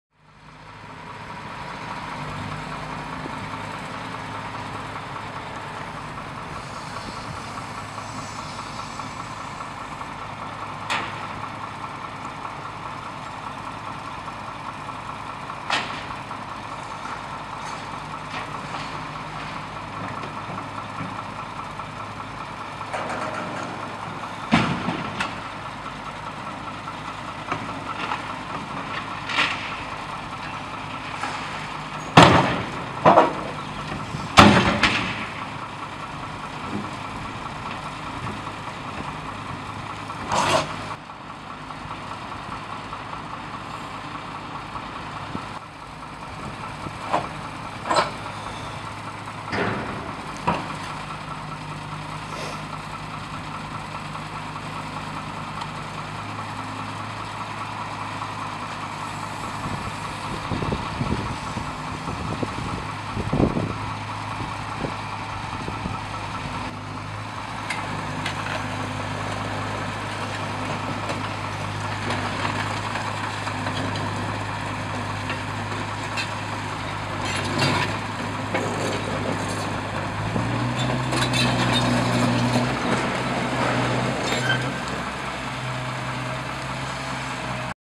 Felbermayr Raupenbagger Liebherr R 916
Felbermayr Liebherr R 916 Raupenbagger beim Abbruch eines Geschäftshauses in Wels,Österreich.